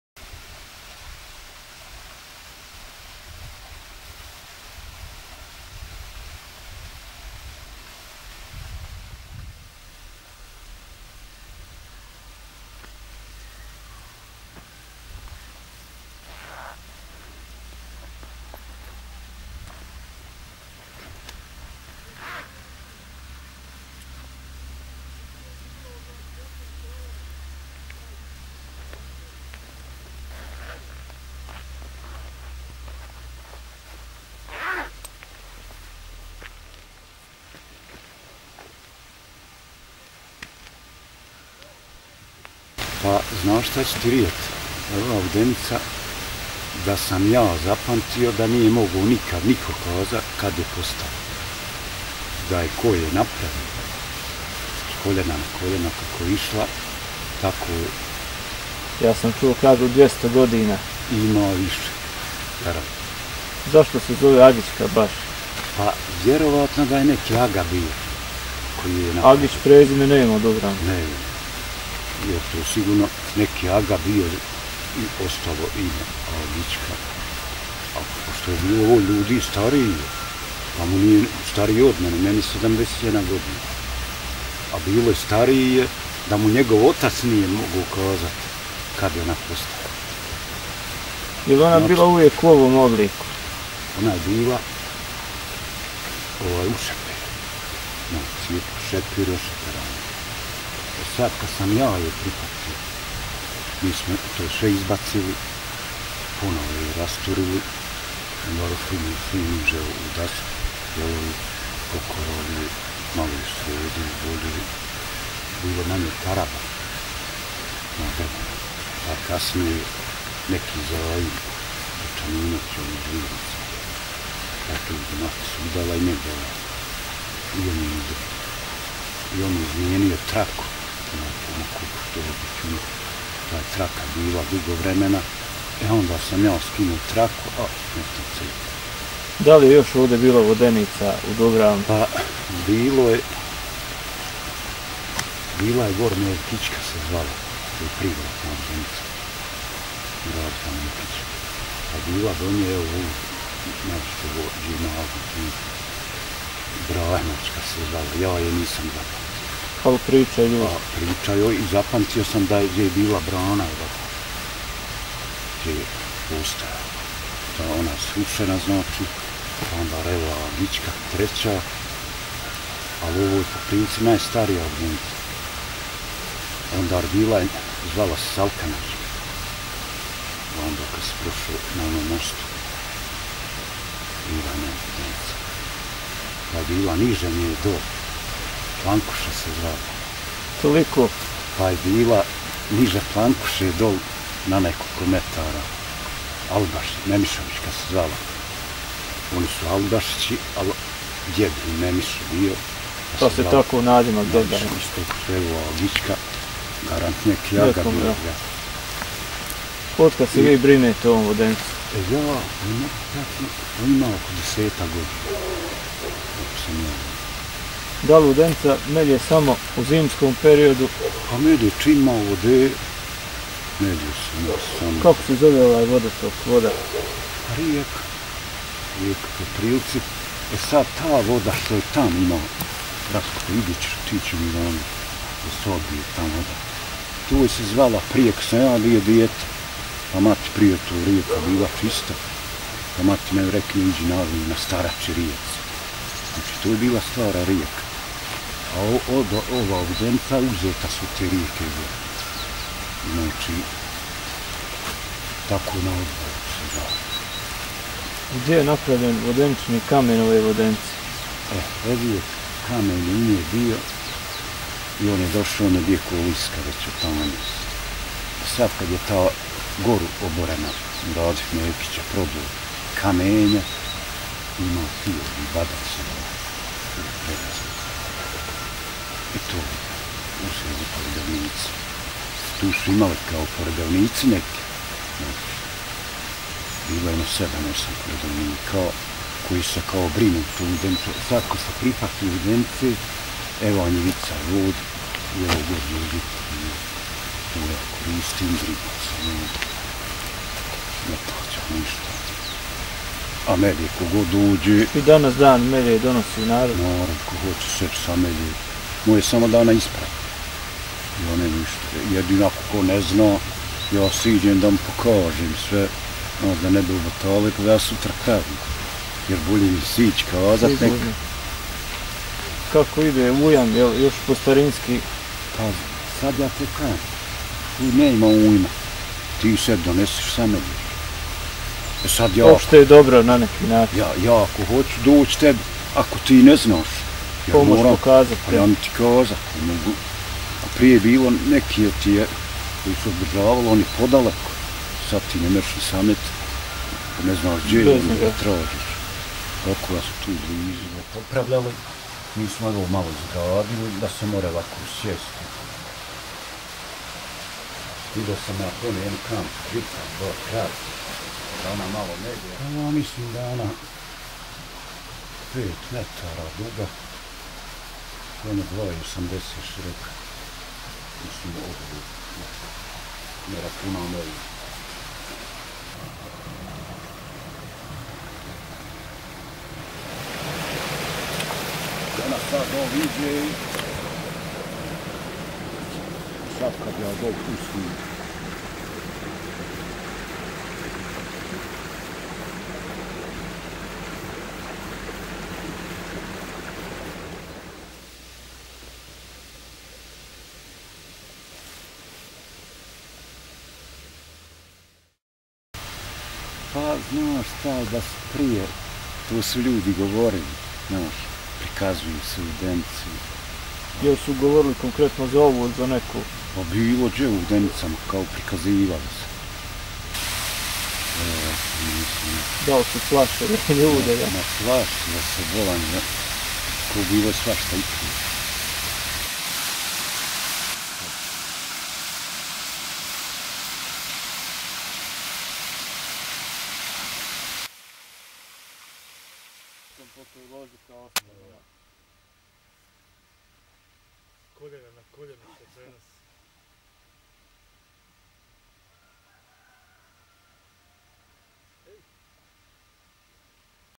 Vodenica Agićka- Gornje Dubrave
Vodenica kojom se brine Himzo Dedić (71), a ona predstavlja opšte dobro, jer u njoj melje svako ko želi, i tu ne postoji ujam. Ovo je i mjesto okupljanja ljudi, a o starosti vodenice niko sa sigurnošću ne može posvjedočiti. Zanimljiv je i aspekt običajnog prava da onaj ko se brine o vodenici koristi zemljišnu parcelu koja je okružuje.